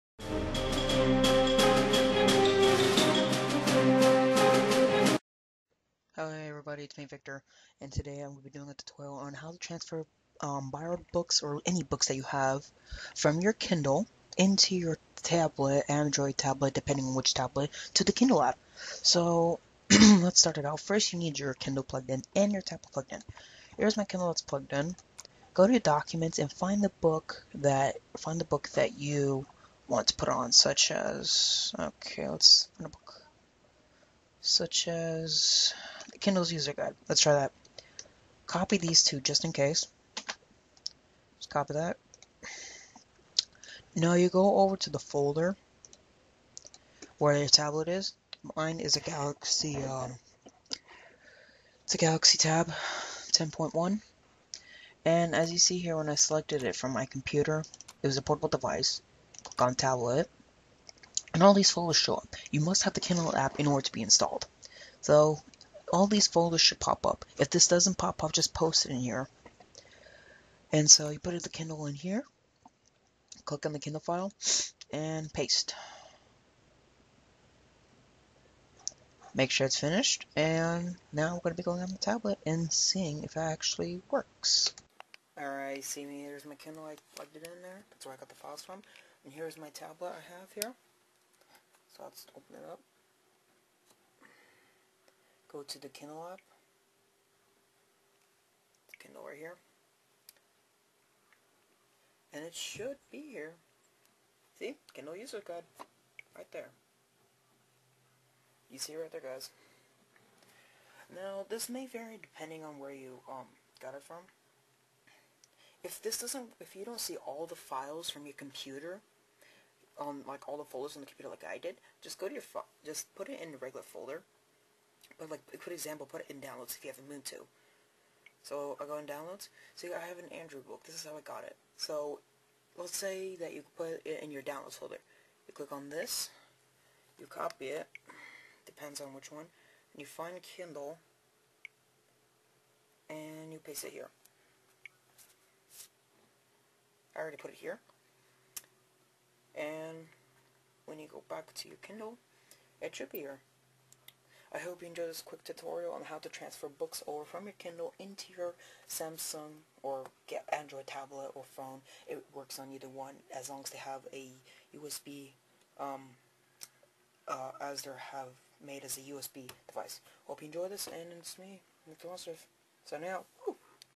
Tips & Tricks: How to Transfer Books from a Kindle to a Kindle App on Android
This is just a simple, straight up trick on how to transfer books from your Kindle to your Kindle App on your Android device